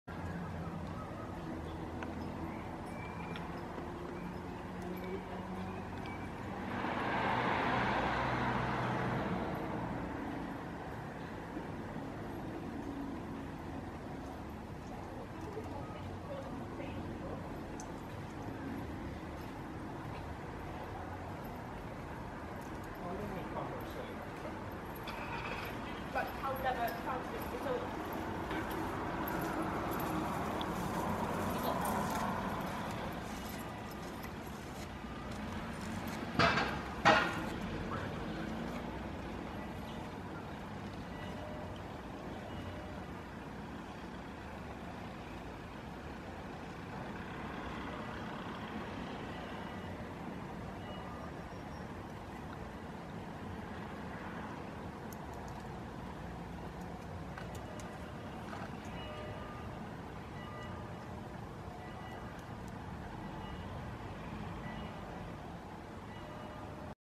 Drone over Swansea Sea front
Remote controlled drone spotter hovering above Swansea seafront beach. S-Wales, United Kingdom.